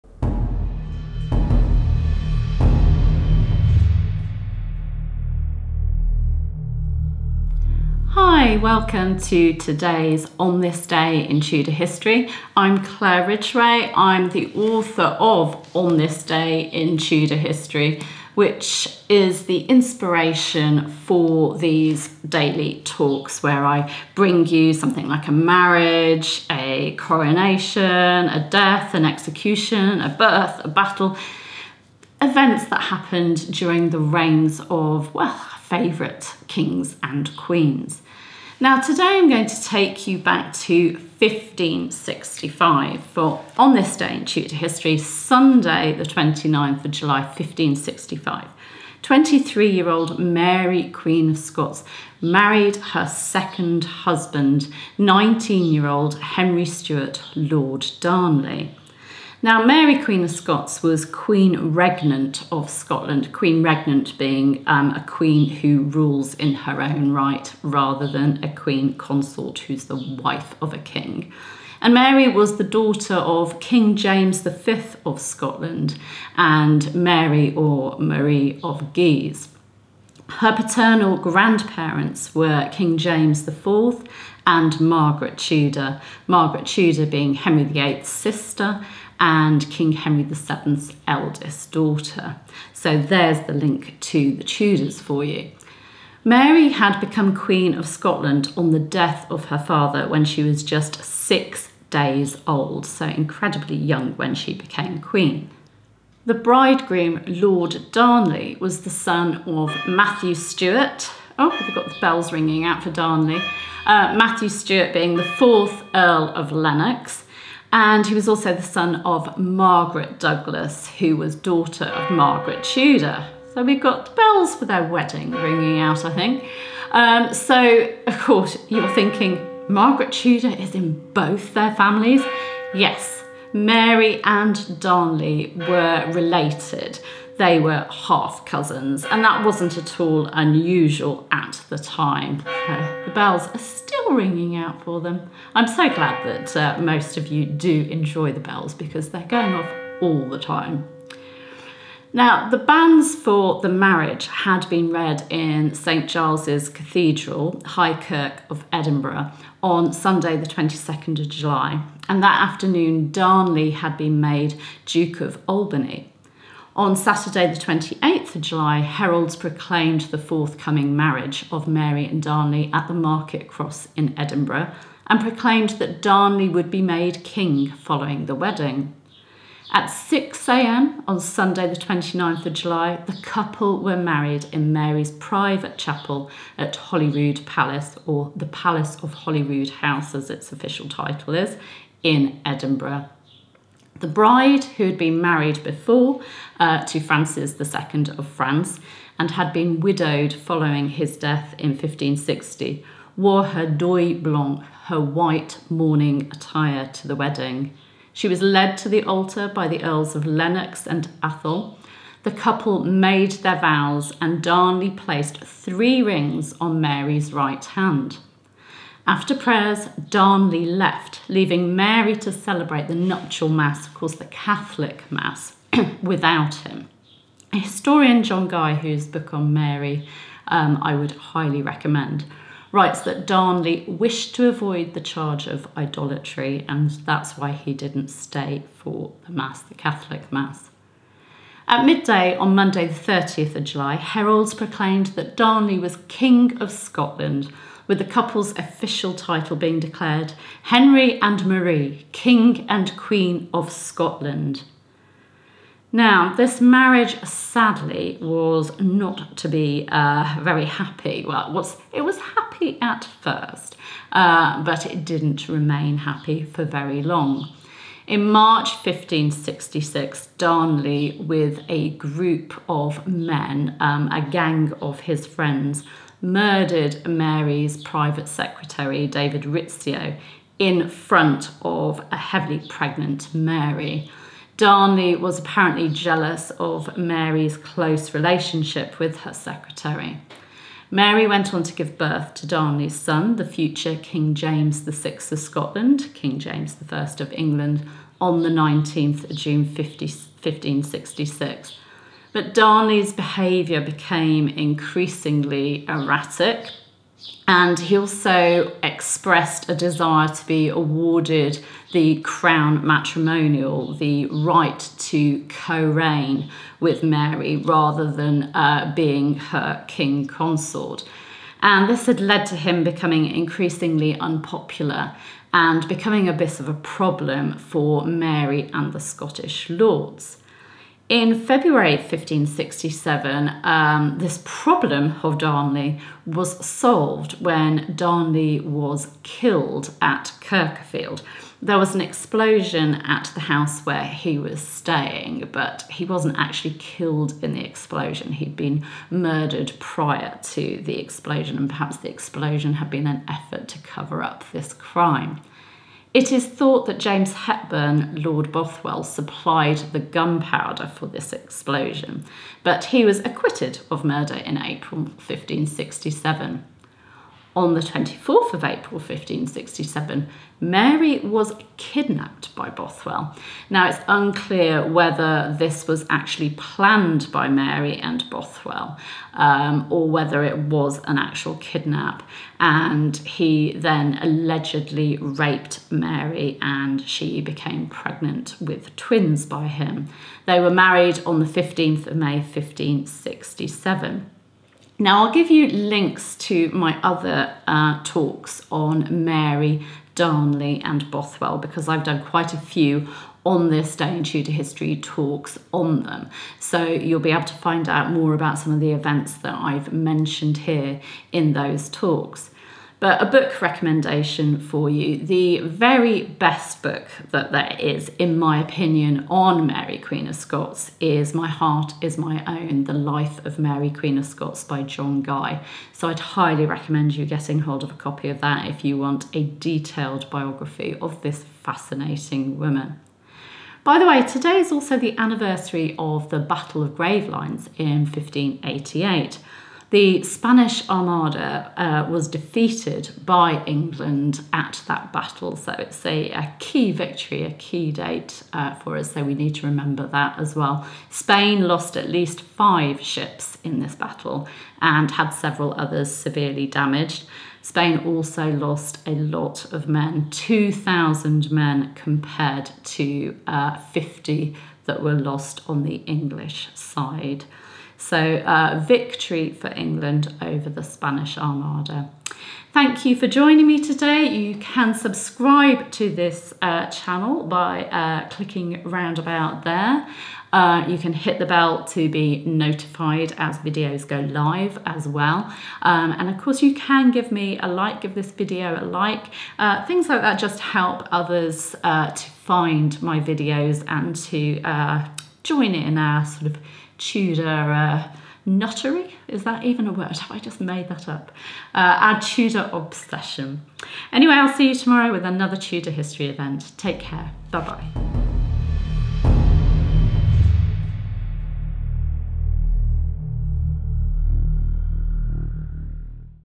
July 29 - Mary, Queen of Scots gets married
29 July 1565: Mary, Queen of Scots Marries Lord Darnley 💍👑

On this day in Tudor history, 29th July 1565, 23-year-old Mary, Queen of Scots, married 19-year-old Henry Stuart, Lord Darnley, in a private chapel at Holyrood Palace. 👑💕

This royal union started with high hopes but quickly turned tragic. Discover the fascinating and tumultuous story of their marriage and the dramatic events that followed. 📜🔥

Learn more about the Queen, her new husband, and the scandal that rocked Scotland. Don't miss it! 🎥👀

April 14 - The death of the insane Earl of Bothwell, husband of Mary, Queen of Scots

Book Recommendation: My Heart is My Own by John Guy.